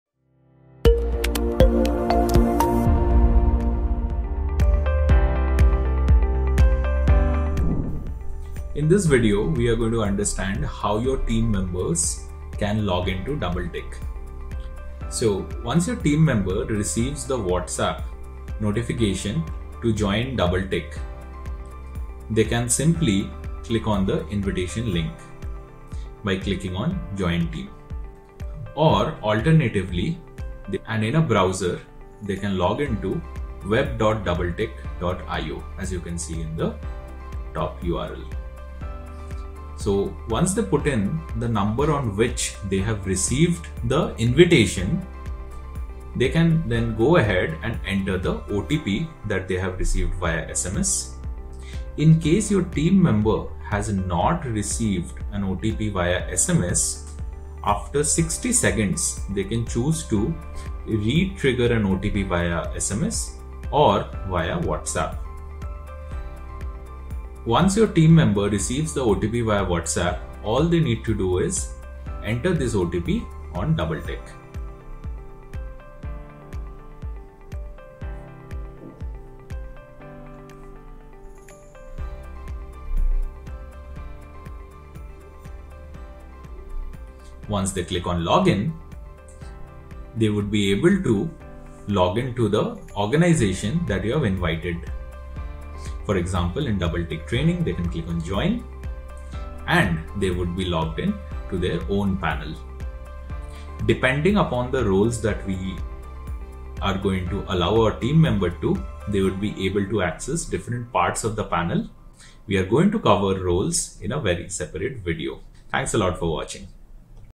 DoubleTick WhatsApp API - Accept Invitation & Login Guide
In this tutorial, we'll walk you through the process of accepting invitations and logging in to DoubleTick WhatsApp API effortlessly. DoubleTick is your go-to solution for seamless WhatsApp integration. Learn how your team members can quickly accept invitations and access the powerful features of DoubleTick. Simplify your communication strategy with DoubleTick WhatsApp API today!